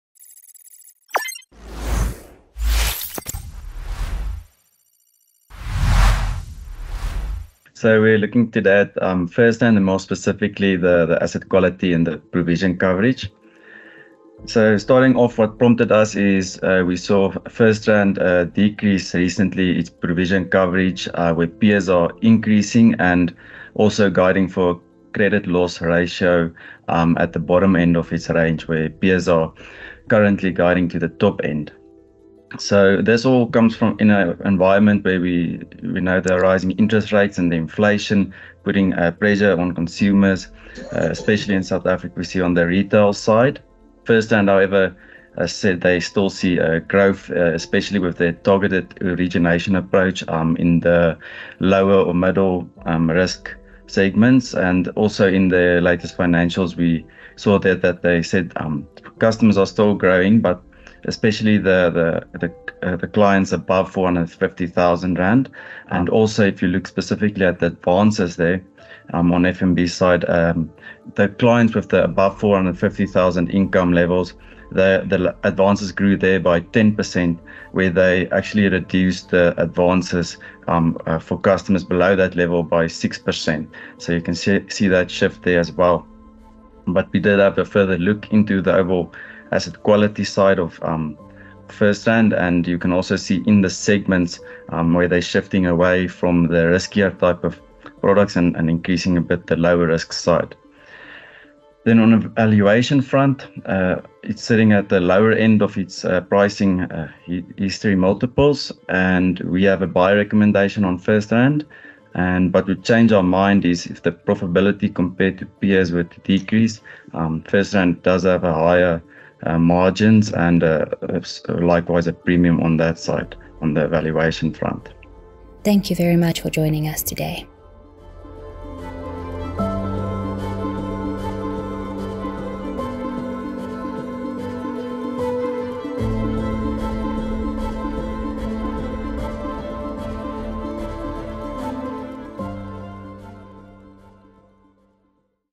Investment idea: FirstRand
- FirstRand reduced its loan loss provision coverage, and its credit loss ratio (CLR) is at the lower end of its through (through-the-cycle ((TTC) range which stands in contrast to peers with ratios at or above the top of their ranges. This prompted us to investigate and brought the following insights:
1. We deem the coverage to be adequate due to:
a. Current provision coverage above 3% remains at historically high levels.
b. Non-performing loans are improving, although still lower than historical highs such as during the global financial crises when FirstRand had a lower provision coverage.
c. Higher risk stage 2 and 3 loan contributions decreasing from 14% in December 2020 to 12% in December 2022.
d. Core lending mix increased towards lower CLR advances.
e. Management making use of a targeted origination approach which is evidenced by the above improvements in credit quality.
- Our research supports an intrinsic value of R90 per share (upside of 27%) and we recommend a buy position.
- Please join us on Thursday at 11h30 for our Microsoft Teams Q&A sessions.